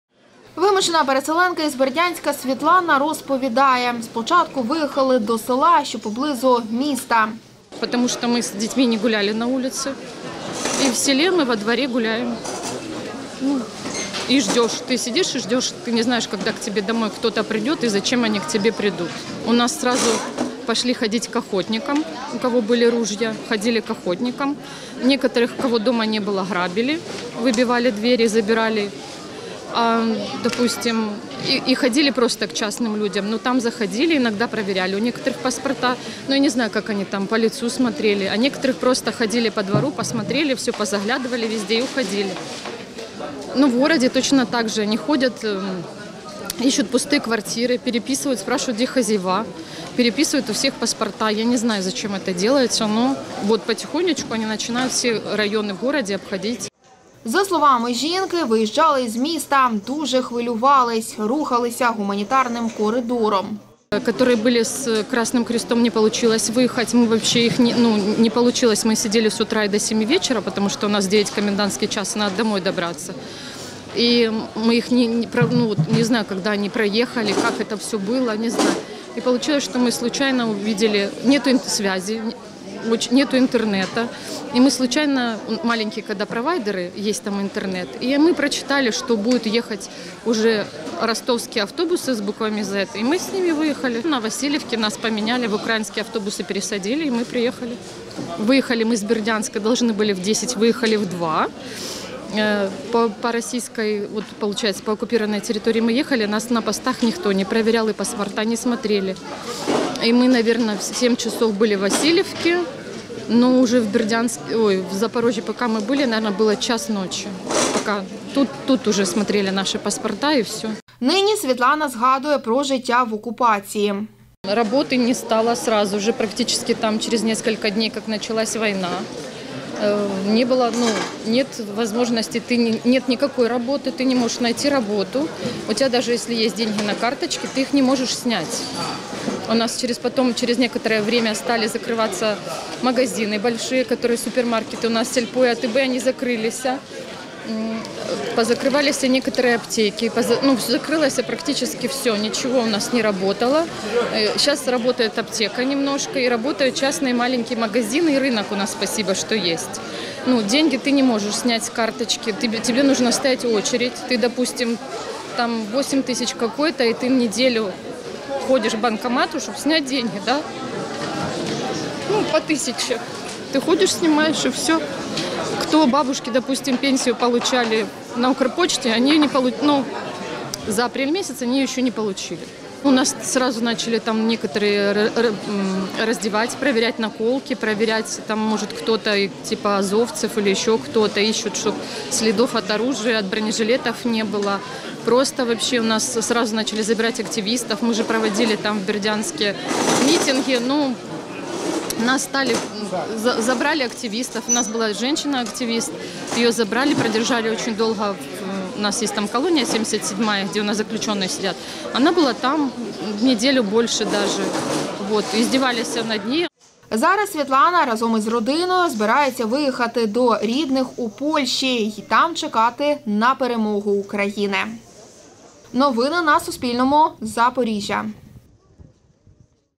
Евакуйована з Бердянська жінка про життя в окупації | Новини
Світлана, разом із п’ятирічним сином та племінницями, евакуювались до Запоріжжя з окупованого Бердянська 3 травня. Каже: виїхати звідти не могли впродовж місяця. Подробиці розповіла кореспондентам Суспільного.